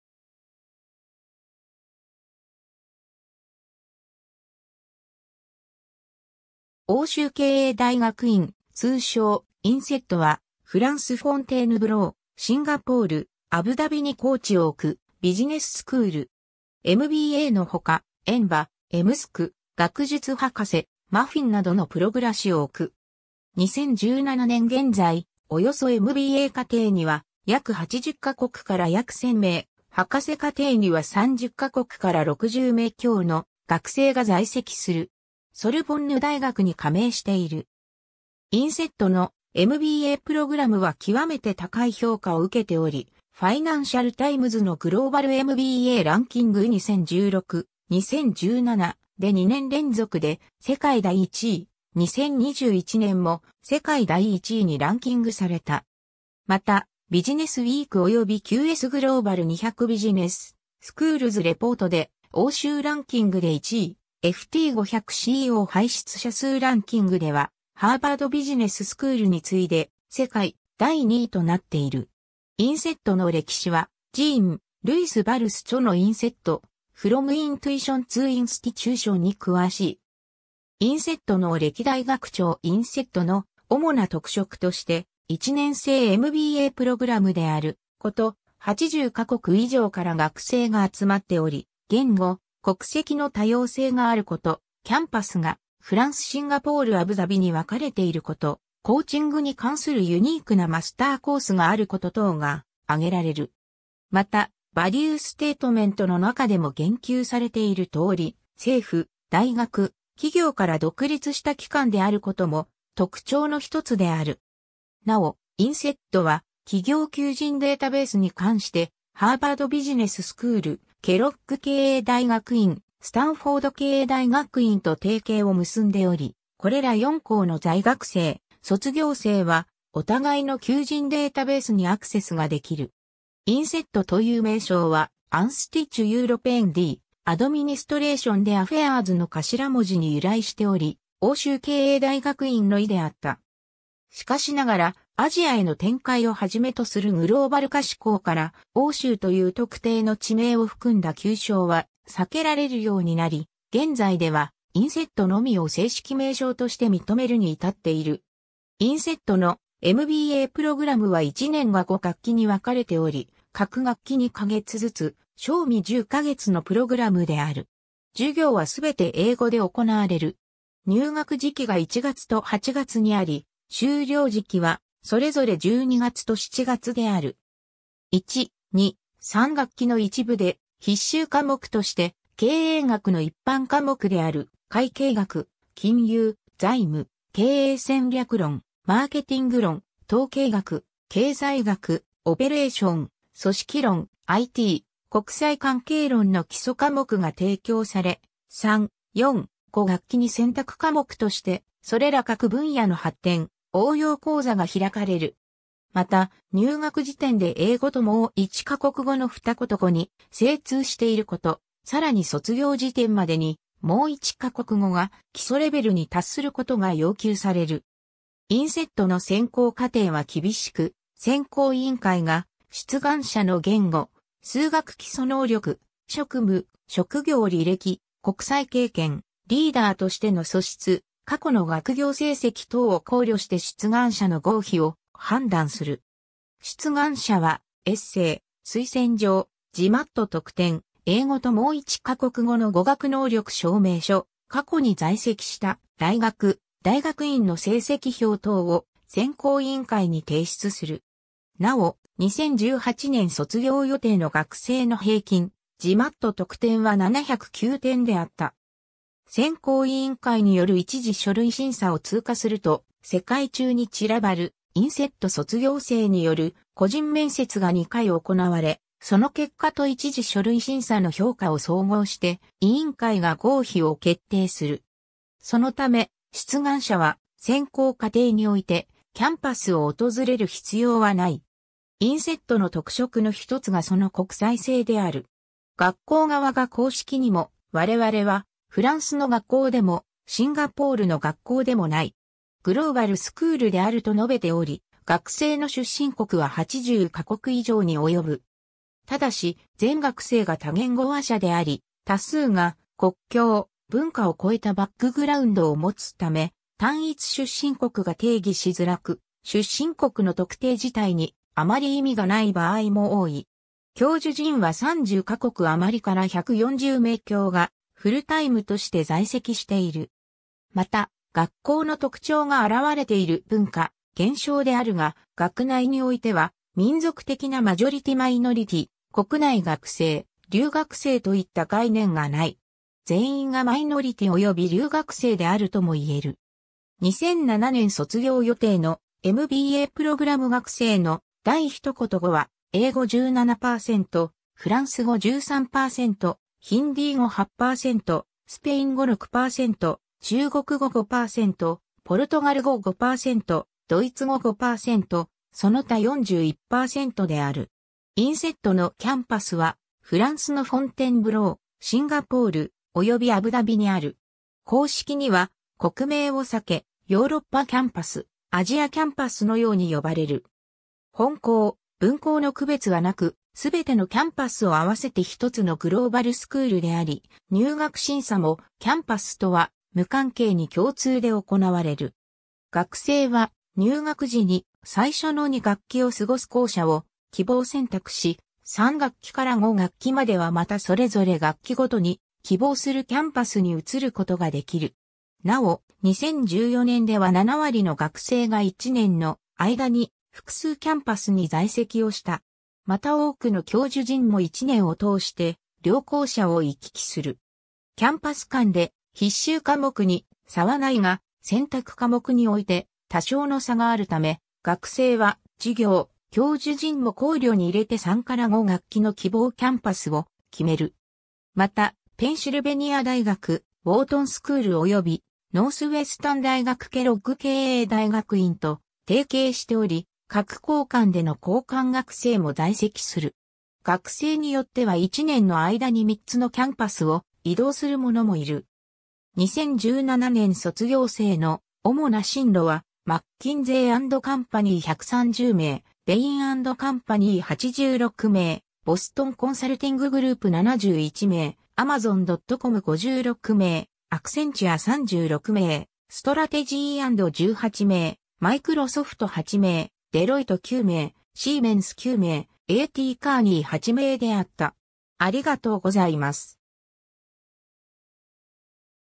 INSEAD
セーヌ＝エ＝マルヌ県
欧州経営大学院（おうしゅうけいえいだいがくいん、フランス語正式名称: Institut européen d'administration des affaires）、通称INSEAD（インシアード）は、フランス・フォンテーヌブロー、シンガポール、アブダビに校地を置くビジネススクール。
 MBAの他、EMBA（Executive MBA、エグゼクティブMBA）、EMCCC（Executive Master in Consulting and Coaching for Change）、学術博士（PhD）、MFIN（Master in Finance）などのプログラ厶を置く。
 2017年現在、およそMBA課程には約八十ヶ国から約千名、博士課程には三十ヶ国から六十名強の学生が在籍する。
ソルボンヌ大学に加盟している。
 INSEADのMBAプログラムは極めて高い評価を受けており、Financial TimesのGlobal MBA ranking 2016, 2017で2年連続で世界第1位、2021年も世界第1位にランキングされた。
また、ビジネスウィークおよびQS Global 200 Business Schools Reportで欧州ランキングで1位、FT500（時価総額ランキングトップ500社）CEO輩出者数ランキング（Financial Times紙、2017年）では、ハーバード・ビジネス・スクールに次いで世界第2位となっている。
 INSEADの歴史はJean-Louis Barsoux著の"INSEAD: From Intuition to Institution"に詳しい。
 INSEADの歴代学長 INSEADの主な特色として、1年制MBAプログラムであること、80か国以上から学生が集まっており言語・国籍の多様性があること、キャンパスがフランス・シンガポール・アブダビに分かれていること、コーチングに関するユニークなマスターコース（EMCCC）があること等が挙げられる。
また、バリュー・ステートメントの中でも言及されているとおり、政府・大学・企業から独立した機関であることも特徴のひとつである。
 なお、INSEADは企業求人データベースに関して、ハーバード・ビジネス・スクール、ケロッグ経営大学院、スタンフォード経営大学院と提携を結んでおり、これら4校の在学生・卒業生はお互いの求人データベースにアクセスができる。
 INSEADという名称は「Institut Européen d'Administration des Affaires （European Institute of Business Administration）」の頭文字に由来しており、「欧州経営大学院」の意であった。
しかしながら、アジアへの展開を始めとするグローバル化志向から、「欧州」という特定の地名を含んだ旧称は避けられるようになり、現在では「INSEAD」のみを正式名称として認めるに至っている。
 INSEADのMBAプログラムは1年が5学期に分かれており、各学期2か月ずつ、正味10か月のプログラムである。
授業は全て英語で行われる。
入学時期が1月と8月にあり、修了時期はそれぞれ12月と7月である。
1、2、3学期の一部で必修科目として、経営学の一般科目である会計学、金融・財務、経営戦略論、マーケティング論、統計学、経済学、オペレーション、組織論、IT、国際関係論の基礎科目が提供され、3、4、5学期に選択科目としてそれら各分野の発展・応用講座が開かれる。
また、入学時点で英語ともう1か国語(英語以外の母語でも可)の二言語に精通していること、さらに卒業時点までに、もう1か国語が基礎レベルに達することが要求される。
 INSEADの選考過程は厳しく、選考委員会(Admissions Committee)が出願者の言語・数学基礎能力(GMAT)、職務・職業履歴、国際経験、リーダーとしての素質、過去の学業成績等を考慮して出願者の合否を判断する。
出願者はエッセイ、推薦状、GMAT得点、英語(TOEFL)ともう1か国語の語学能力証明書、過去に在籍した大学・大学院の成績表等を選考委員会に提出する。
なお、2018年卒業予定の学生の平均GMAT得点は709点であった。
 選考委員会による一次書類審査を通過すると、世界中に散らばるINSEAD卒業生による個人面接が2...